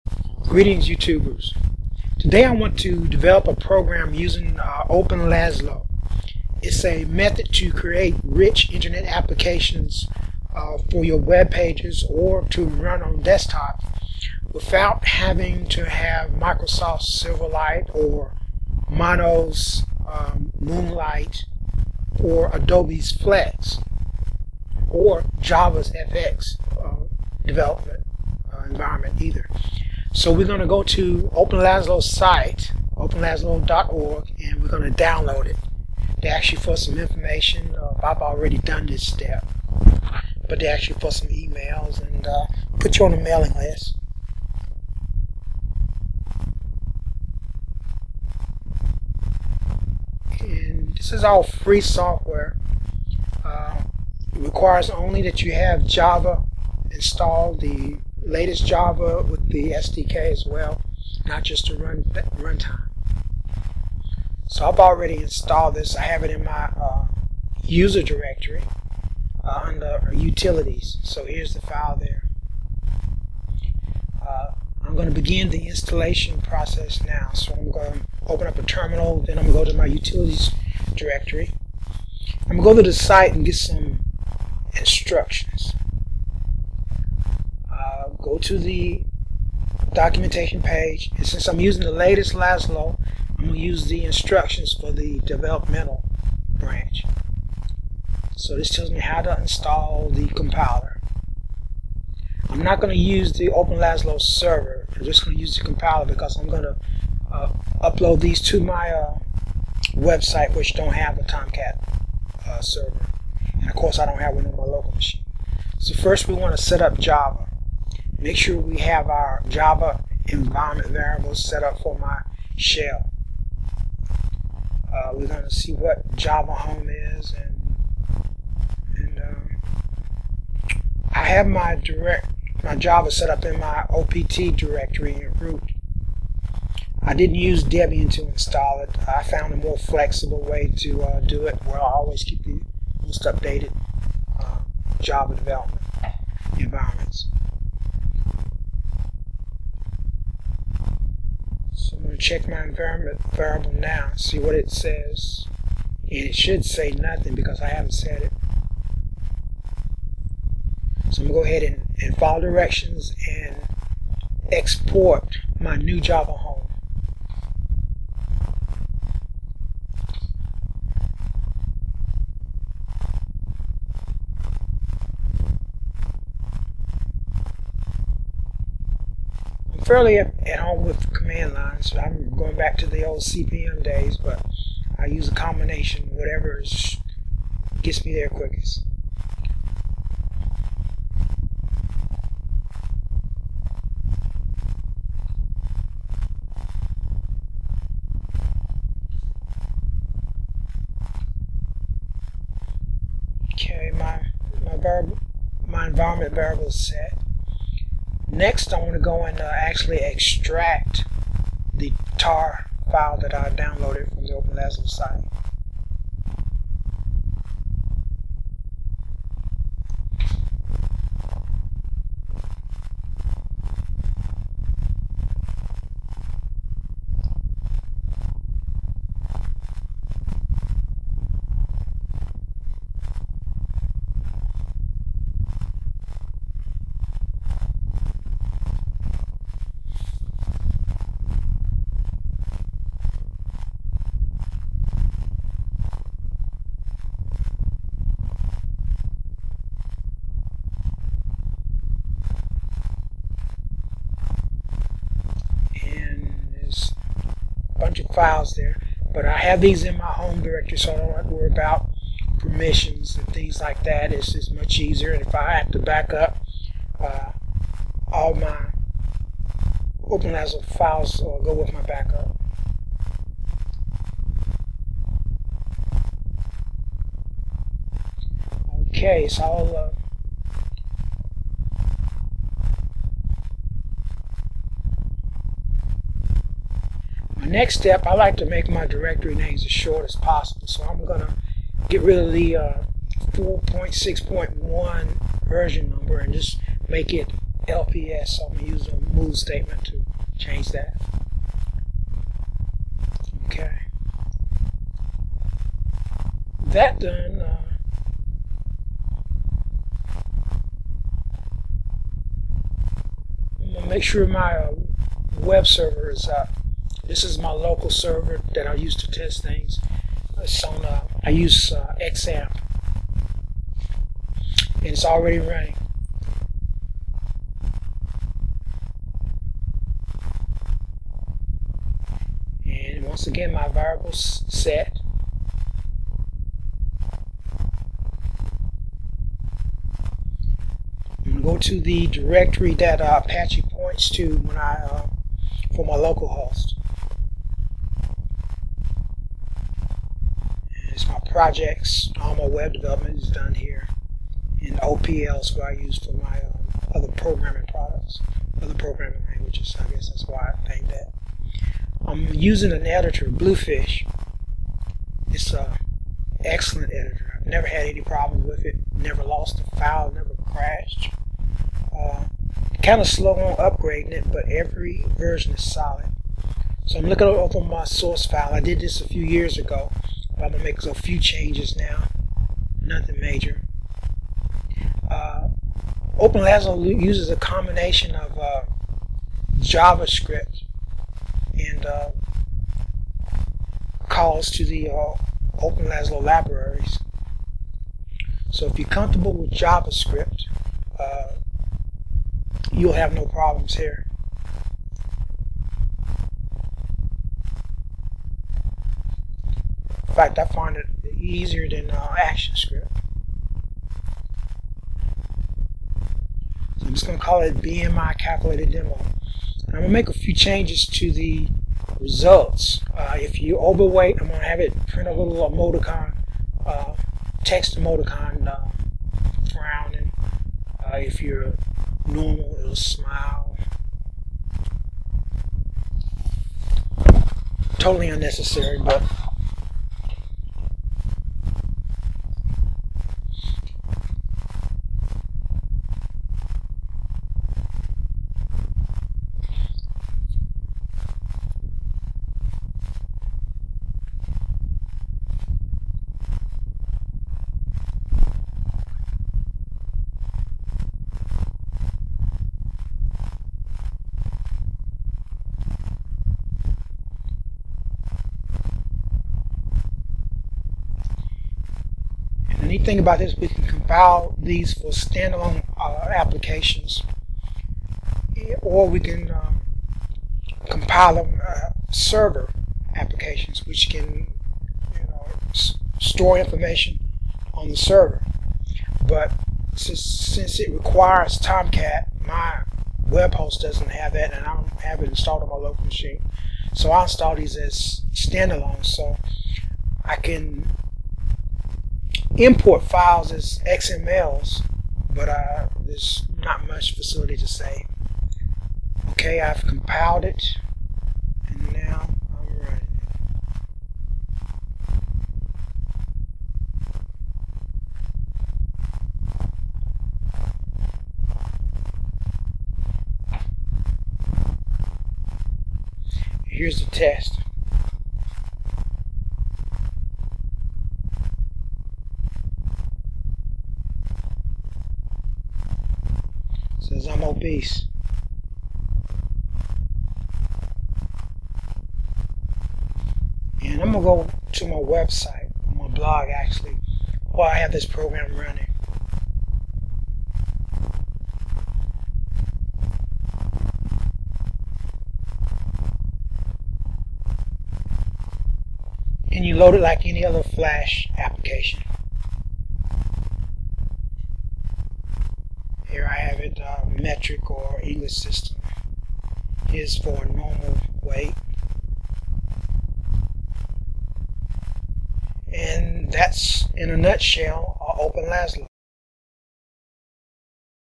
Using Openlaszlo to Develop Rich Internet Applications
This video provides a brief explanation of how to setup and use IBM's openlaszlo RIA environment to create a simple web application. In this instance, a BMI calculator.

Examples of other  RIA frameworks include Ajax,  Adobe Flash/Adobe Flex/AIR, Java/JavaFX,[2] Mozilla's XUL and Microsoft Silverlight, and mono's Moonlight.

Openlaszlo is free and powerful.

You do need to have a recent Java installed (Including the SDK)